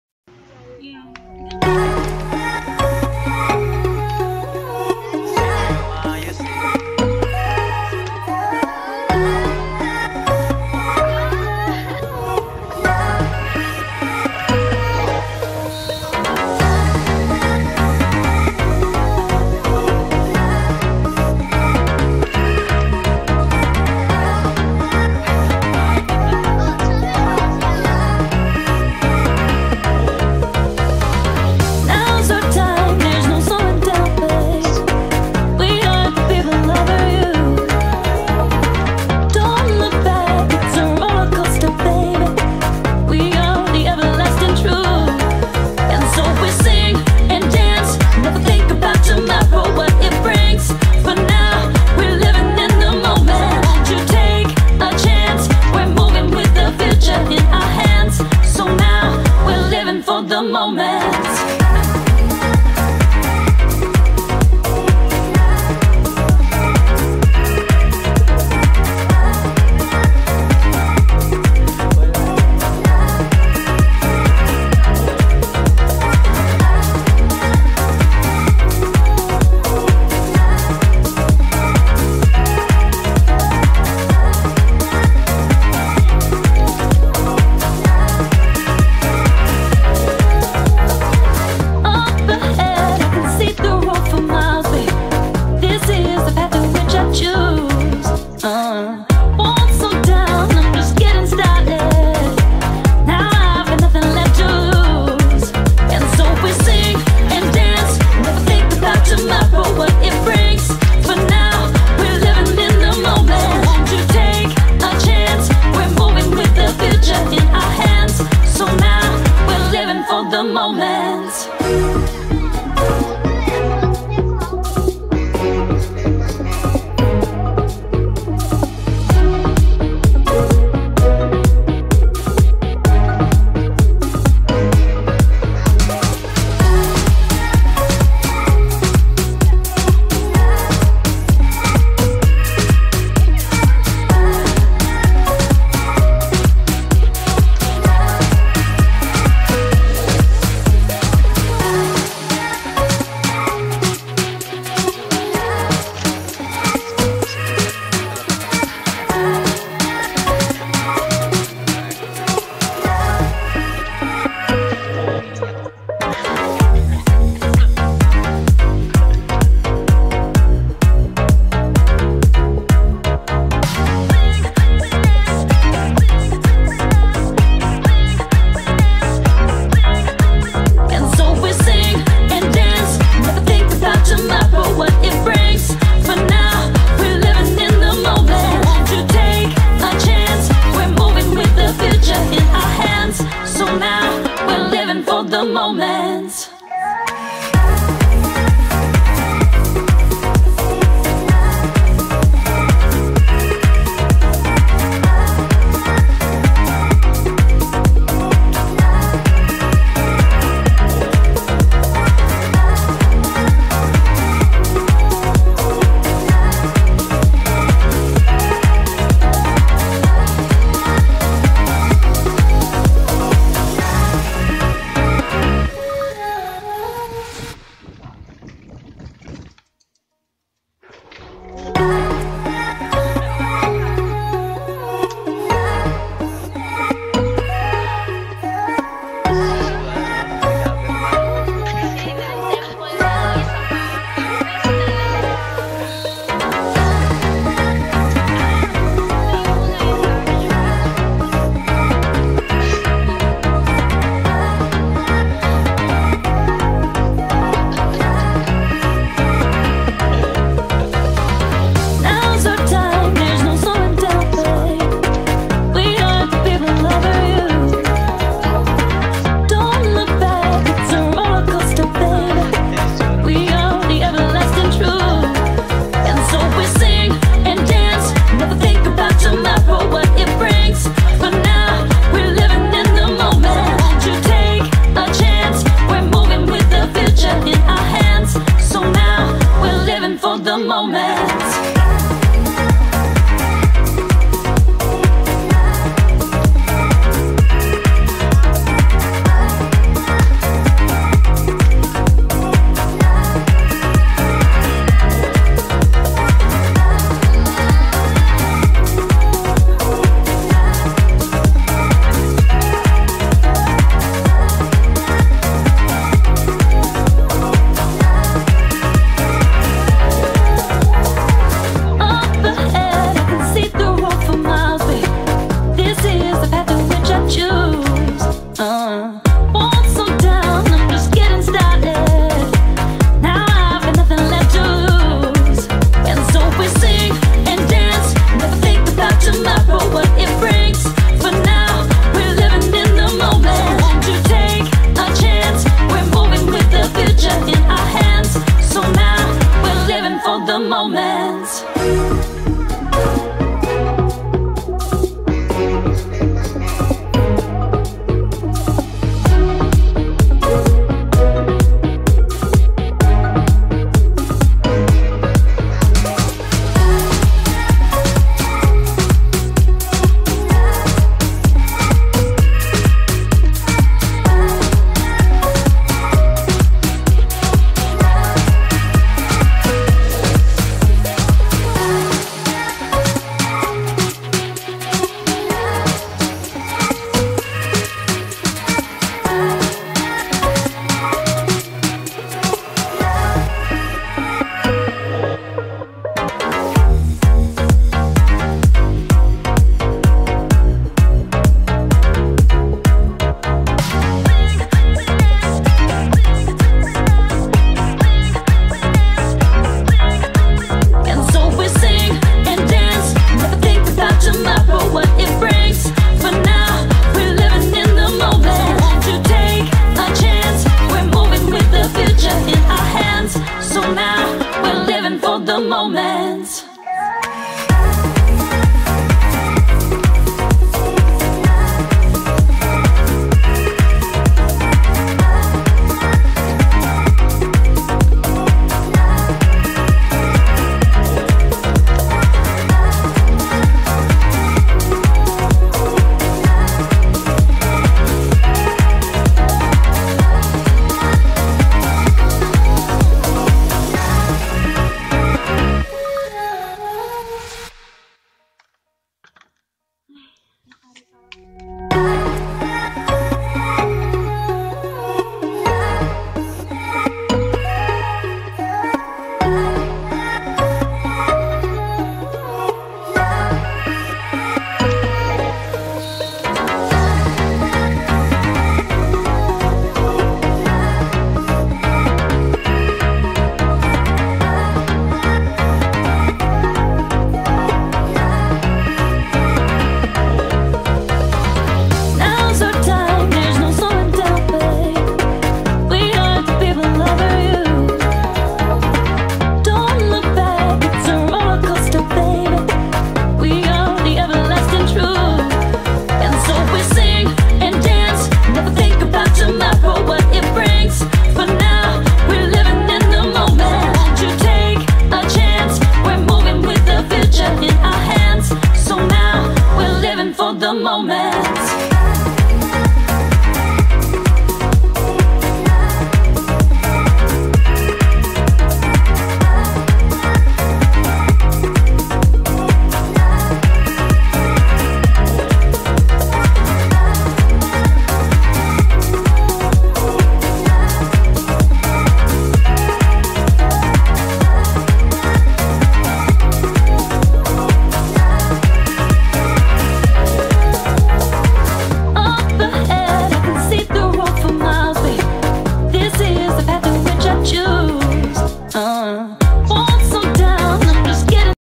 EXPLORING TAIPEI MAOKONG GONDOLA
It was our first experience sumakay ng cable car.At first nakakatakot siya but in the middle  you will difinitely enjoy the ride.This was a video we took last year ago pa.